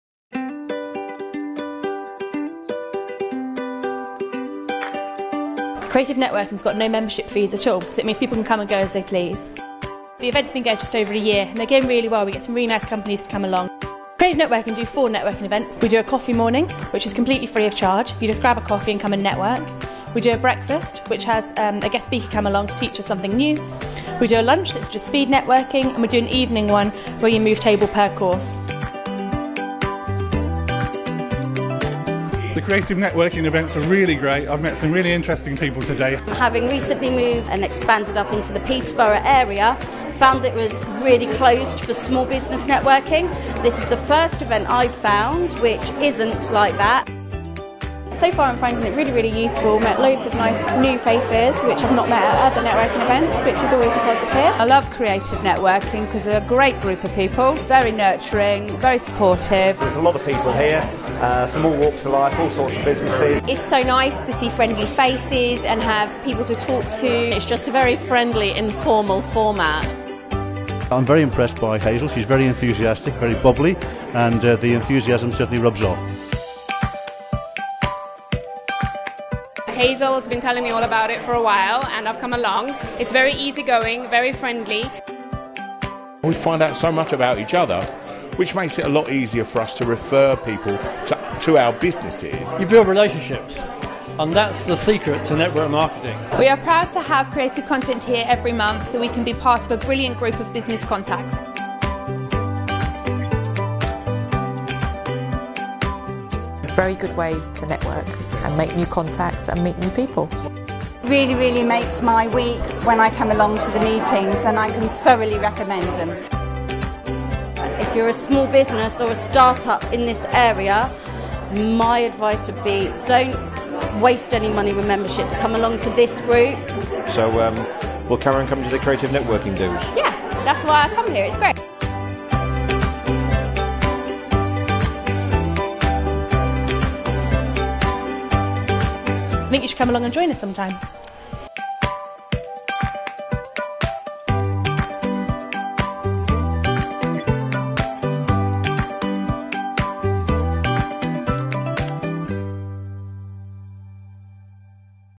Creative Networking | Networking Events In Peterborough
Video for Creative Networking, free networking in Peterborough.
Lovely bunch of people.
Find them on meet up!